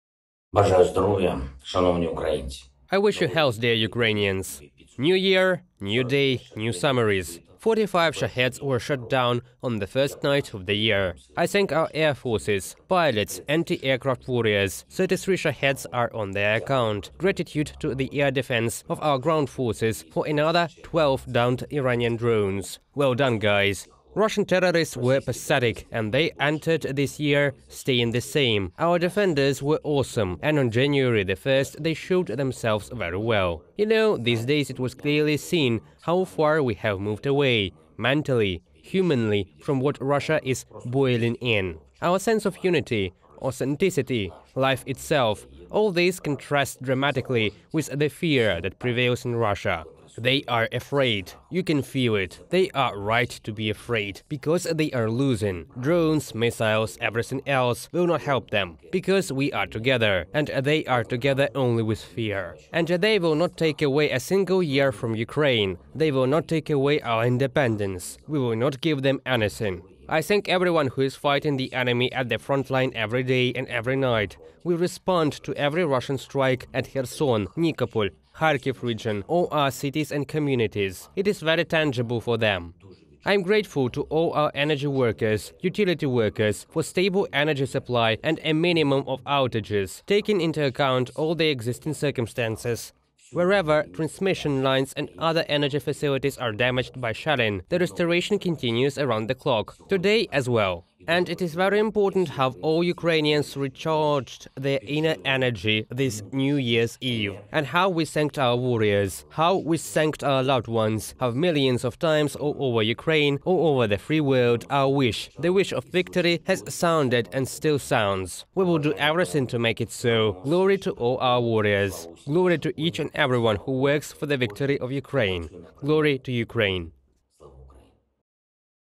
Our sense of unity and authenticity contrasts with what they feel in Russia: with fear – Zelenskyy
'Russian terrorists were pathetic, and they entered this year staying the same. Our defenders were awesome, and on January 1 they showed themselves very well. You know, these days it was clearly seen how far we have moved away, mentally, humanly from what Russia is 'boiling' in. Our sense of unity, authenticity, life itself – all this contrasts dramatically with the fear that prevails in Russia' – the Head of the State said on the evening of January 1 in his daily address.